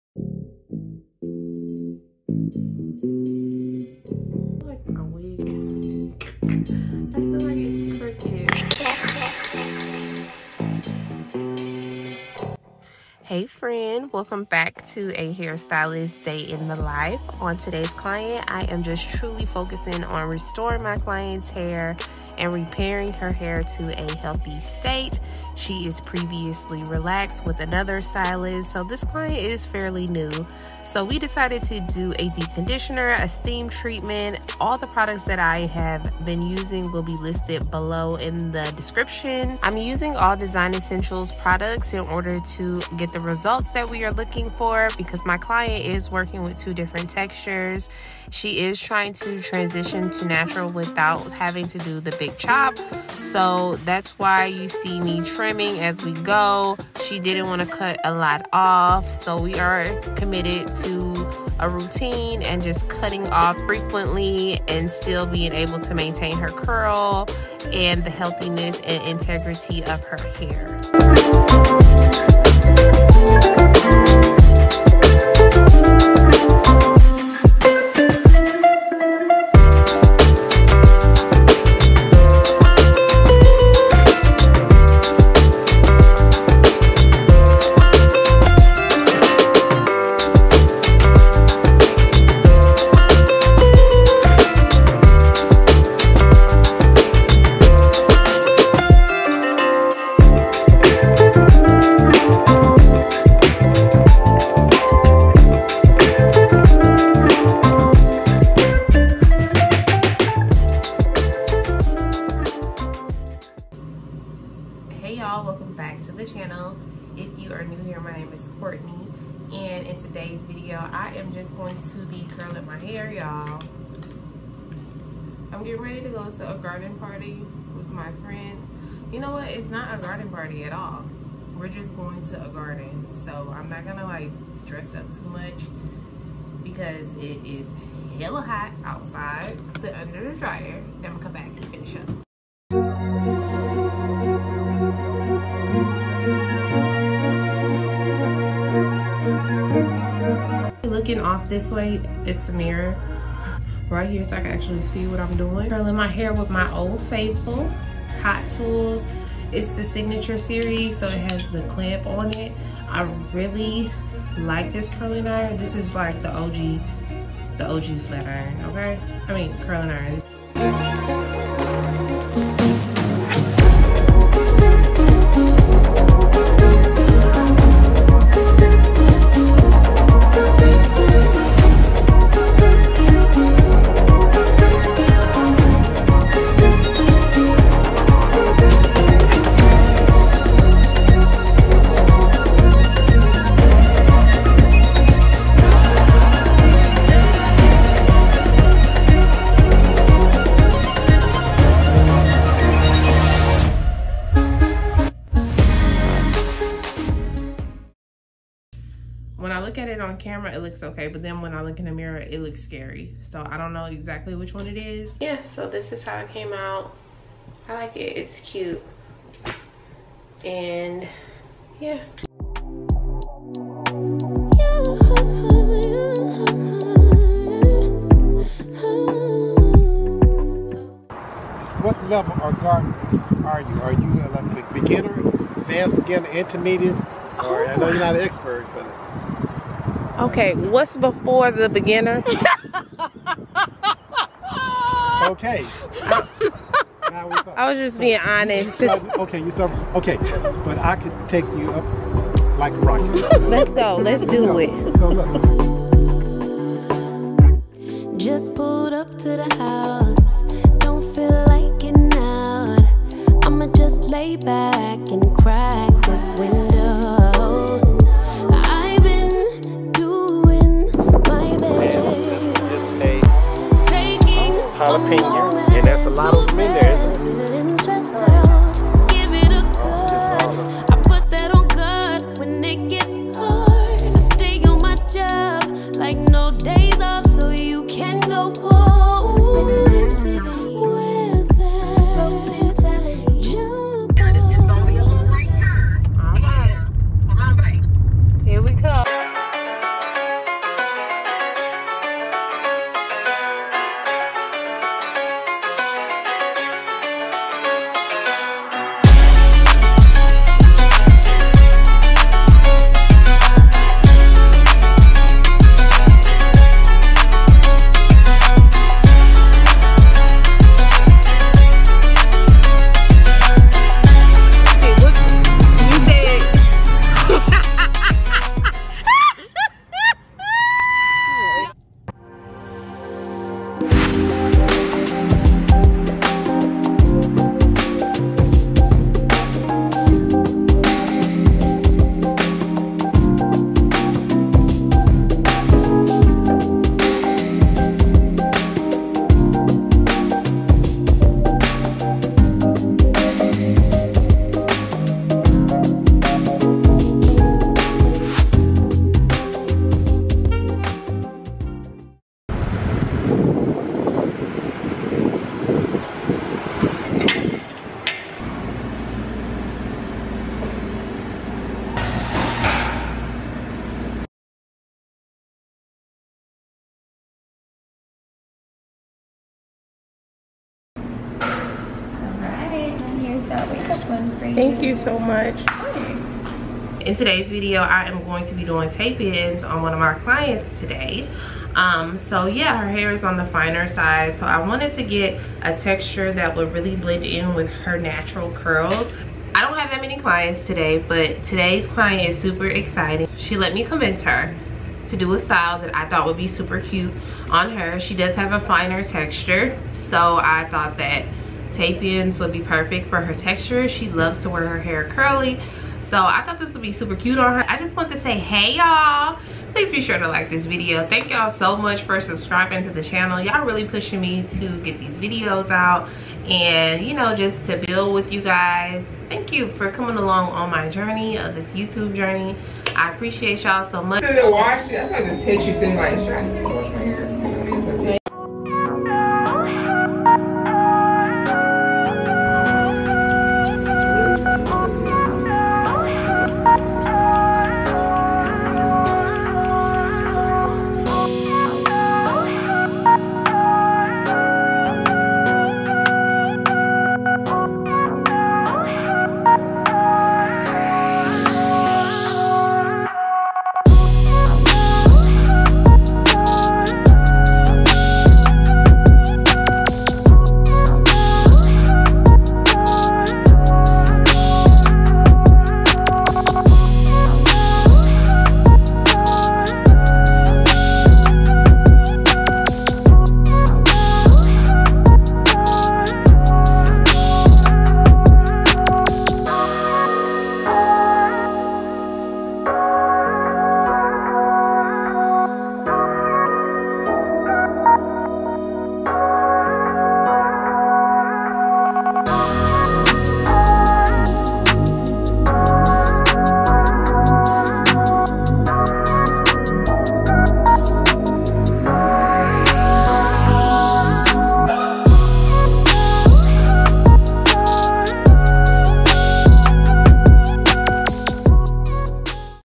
Day In The Life Of A Hair Stylist Vlog | Silk Press on Short Hair| Salon Vlog
Hair Stylist Vlog Day In The Life | Silk Press on Short Hair | Salon Vlog

Products Used:

Blow Dryer

Flat Iron

Heat Protectant

Shampoo and Conditioner

Steamer

Paddle Brush

Heat Resistant Comb